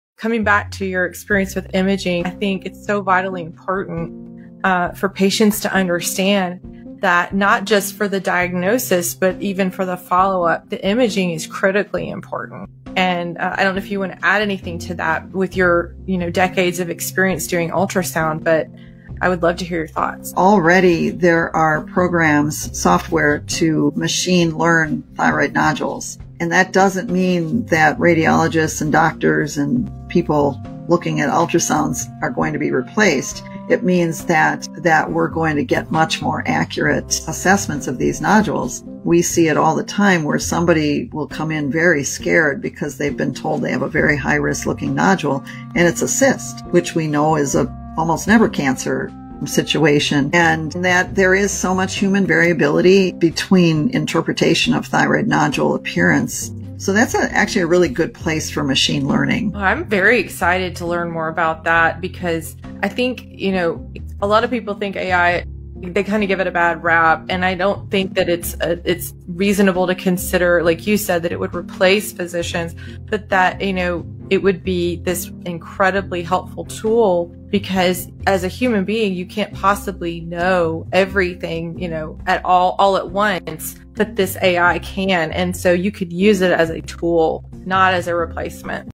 "Someone will come in very scared because they've been told they have a very high risk looking
"Someone will come in very scared because they've been told they have a very high risk looking nodule and it's a cyst."

This common scenario, shared by Dr. Merry Sebelik, highlights a major challenge in thyroid care: human variability in reading ultrasounds can cause unnecessary patient anxiety.

In our new episode, we explore how AI is poised to change that. By providing a more accurate, data-driven assessment of thyroid nodules, machine learning can help prevent the fear that comes from a misinterpretation. It's about using technology to bring patients peace of mind.

🎧 Save Your Thyroid with Jennifer Holkem 🎙️ Episode: A Surgeon's "Wake-Up Call": Dr. Merry Sebelik on the RFA Paradigm Shift 📍 Listen now on YouTube or at saveyourthyroidwithjen.com!